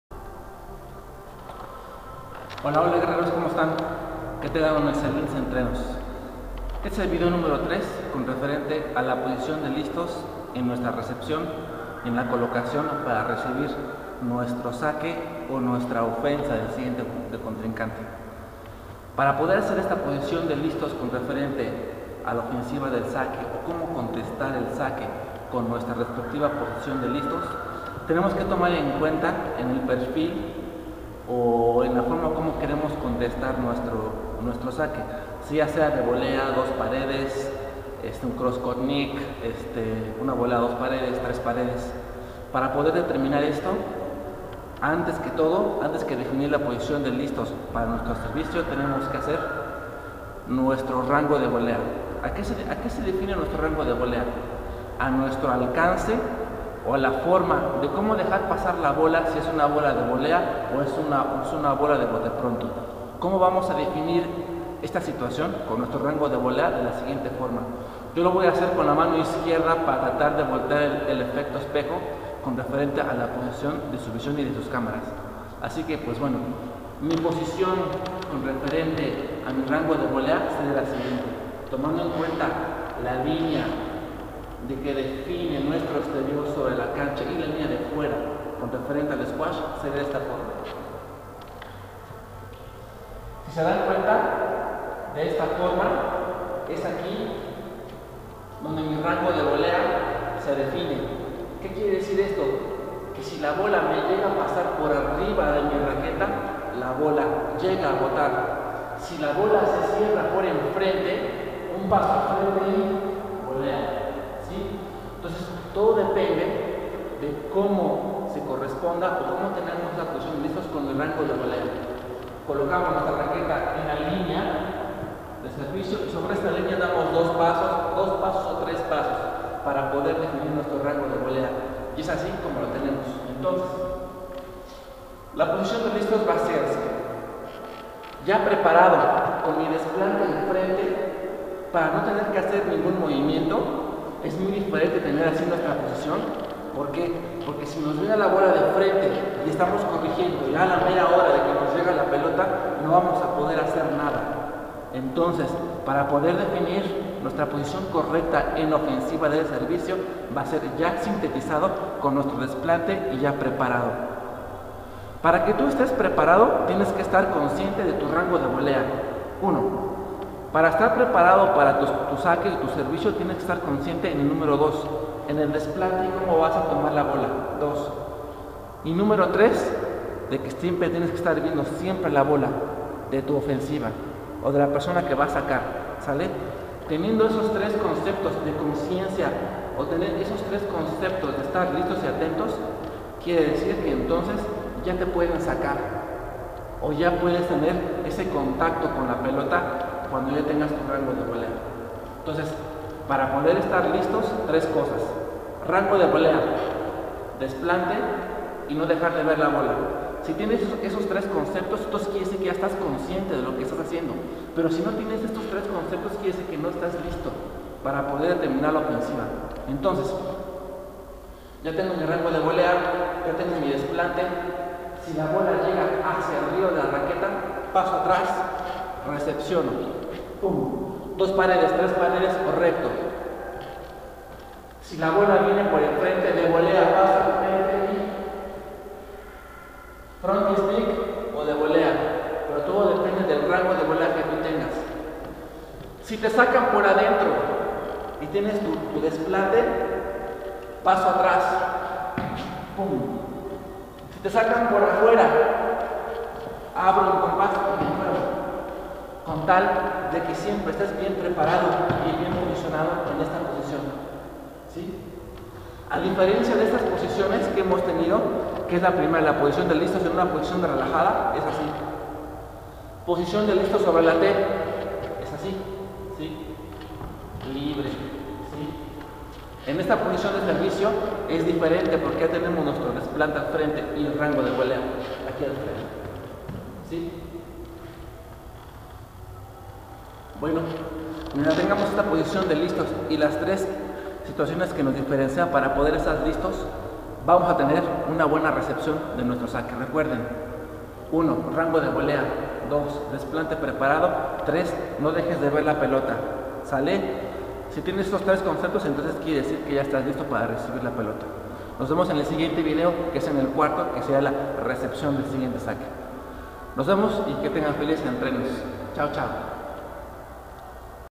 3. Posición de listos para recibir el saque
Posición de recepción de servicio derecho:   ( 1.def, pos de listos)  Esta posición corporal permitirá contestar y contrarrestar la ofensiva de tu oponerte de tal forma que puedas recuperar la posición de listos sobre la T. Ya sea en el desarrollo de un tiro recto de derecha (TRD),  volea de derecha (VD) o 2, 3 paredes dependiendo del estilo y la complejidad del servicio del oponente.